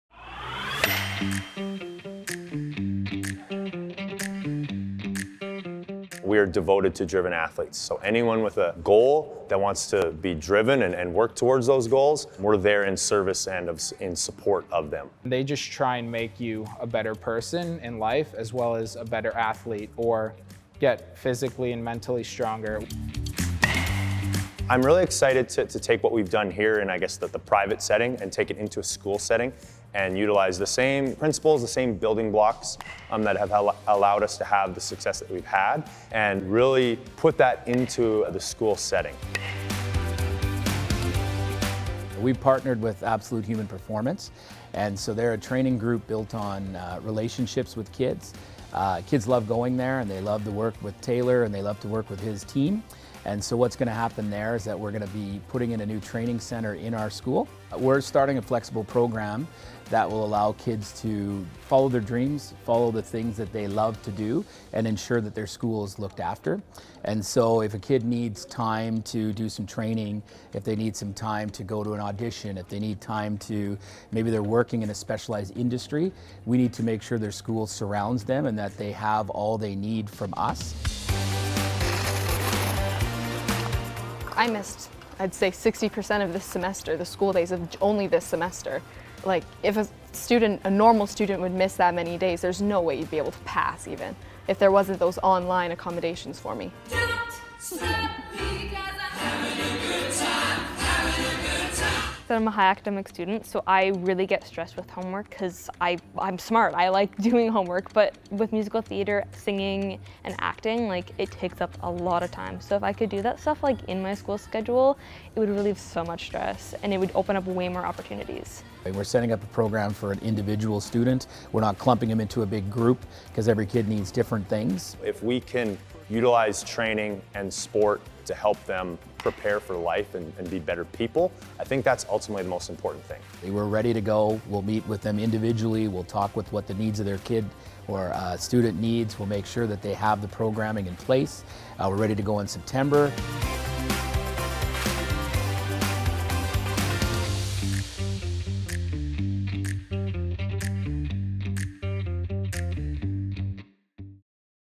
GSACRD - Flexible School Program
Every child is unique, and every child learns differently. GSACRD offers its Flexible Hybrid Program to those students who are looking to put more focus on a specific subject of interest while still maintaining their foundational education. This program caters to individual students with an interest in expanding their education in sports, art, dance, music, theatre, or apprenticeships by shifting their scheduled class time.

Students enrolled in the Flexible Hybrid Program will experience a combination of in-class learning, online learning, and self-learning modules. The structure of this program allows students to dedicate time to their focused discipline while attaining their full education in a structure that works for them.

This program is designed to support students with specific goals for their future and advocate for their success. Each student within the program experiences a personalized timeline to work around their goals while maintaining their educational curriculum. Because of this, students are more in control of their schedules, reducing stress and providing more opportunities to develop skills in their chosen discipline.

GSACRD has introduced a new SACHS Wellness and Training Hub where driven athletes can access equipment and personalized training to develop their skills in a specific sport. On-site training is available to each student enrolled in this program.

Extracurricular activities can demand more time than the traditional family can sustain. Make them part of your child’s daily education by enrolling them in the Flexible Hybrid Program.